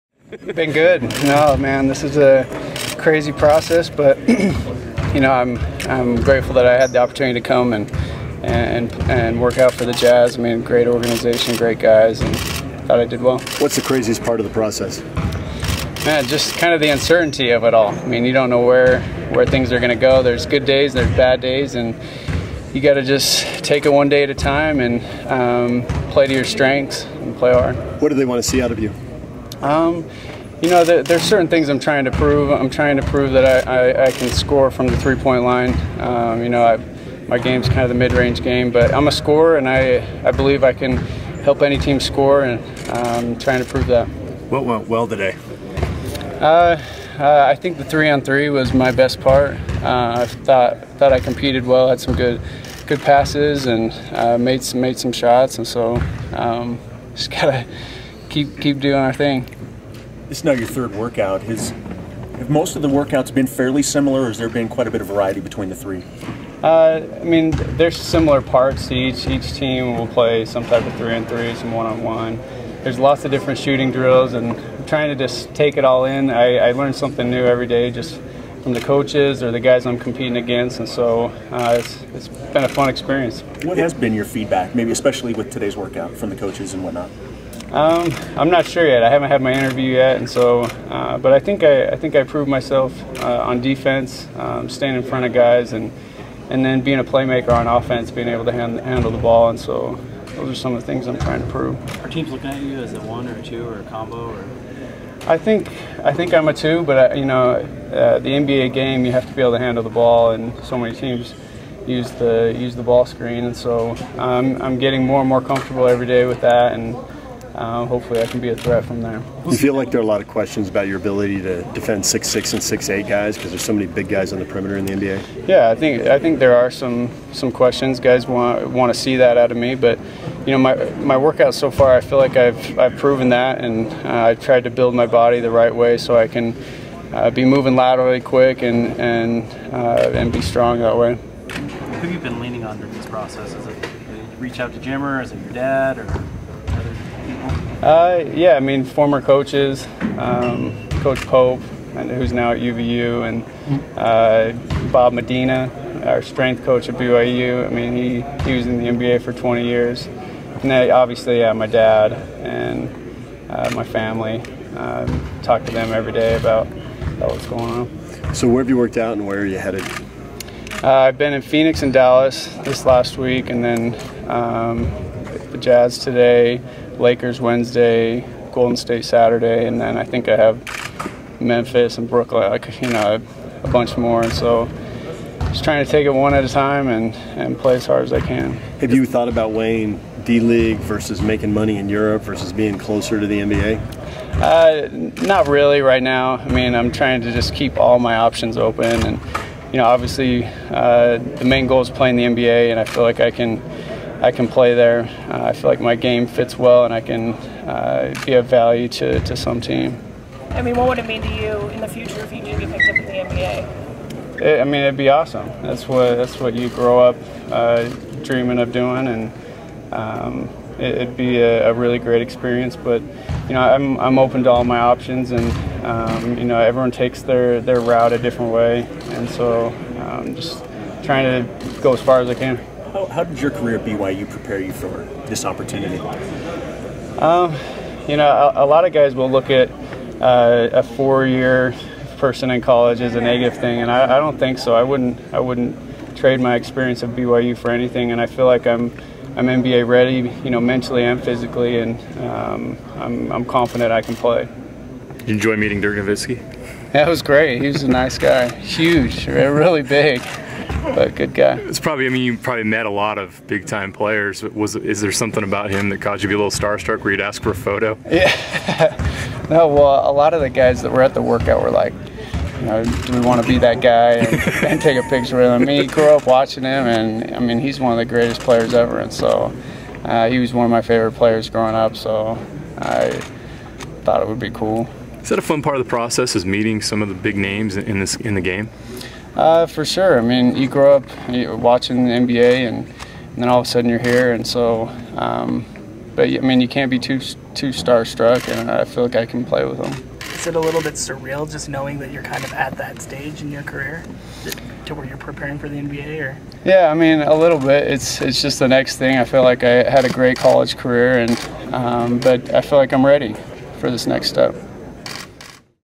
Tyler Haws works out for Utah Jazz
BYU's all-time career scoring leader Tyler Haws talks about his workout with the Jazz, the workout schedule, meeting Dirk Nowitzski and more.